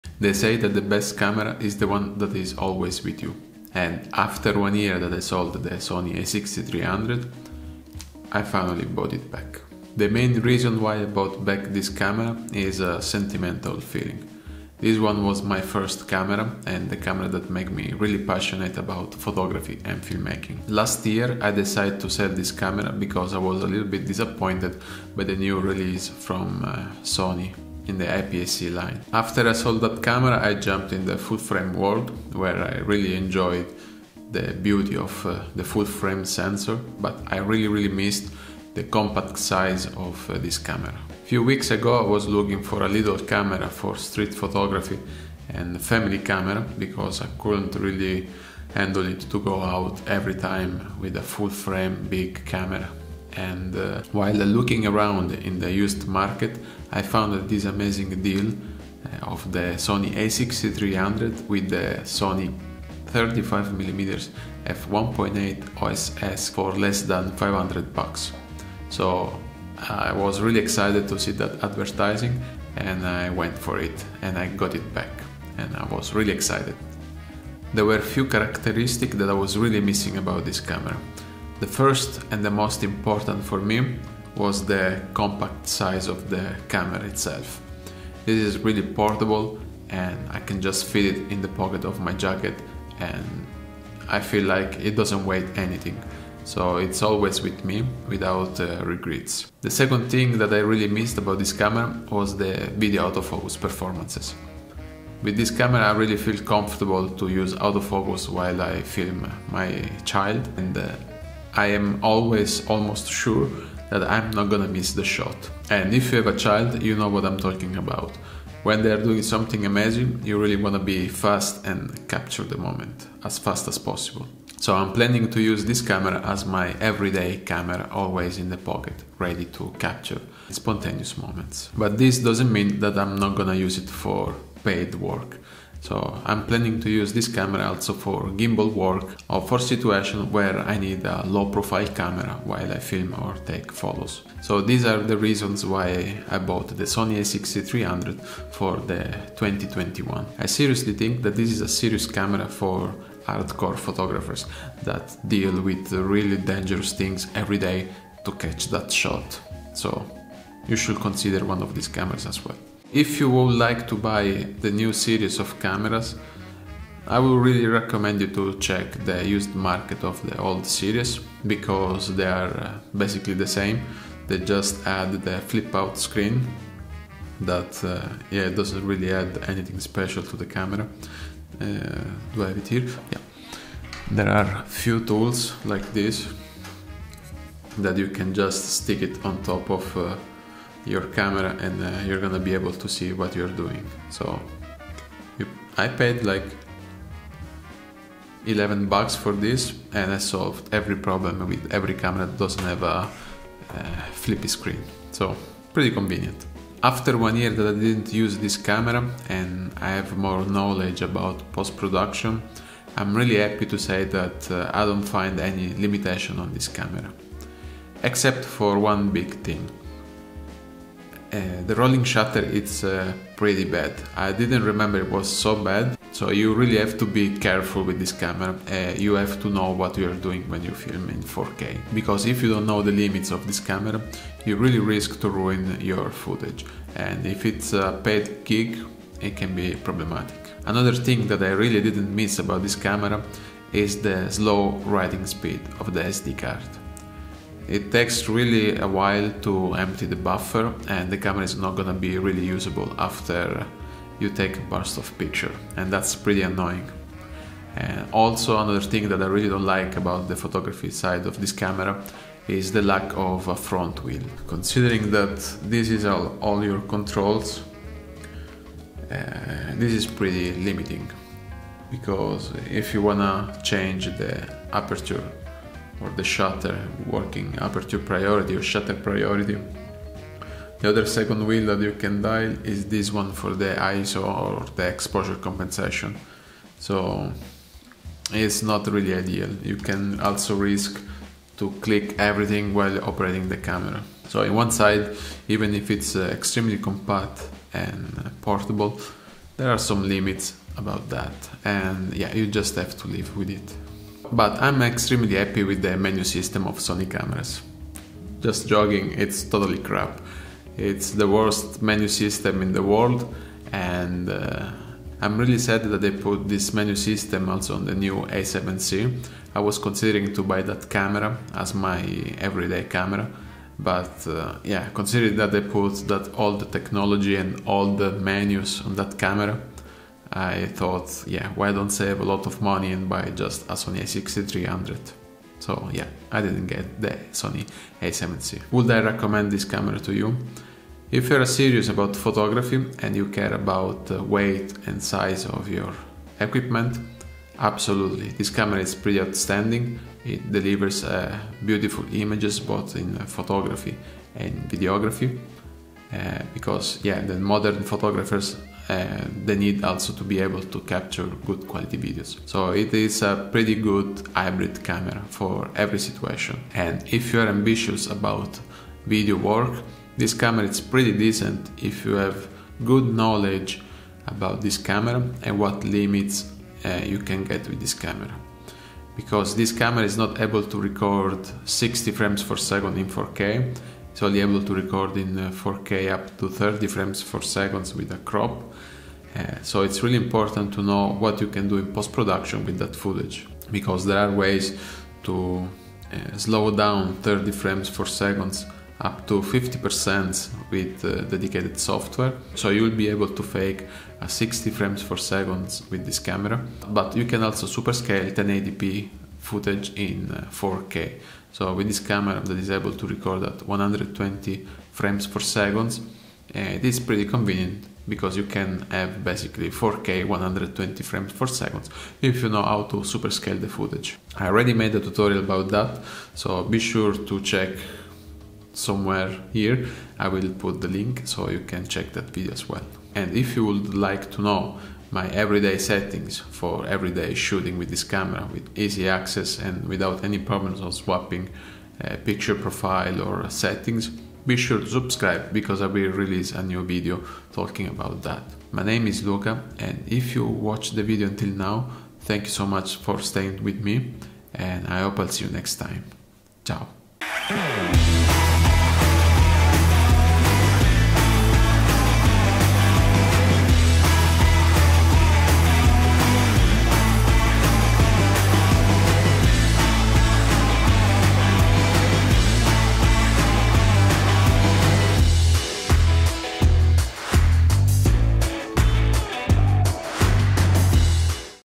Sony a6300 - Still Worth Buying in 2021
Ciao guys, I finally bought back my very first camera after one year that I sold it! in this video I'll explain why I did it, what I missed, what I didn't miss and if this is still a good camera for 2021!

I apologize for my low voice in the video, but my daughter was sleeping while I recorded this video in the night.

- Sony a6400

HOT!  Vlog mirror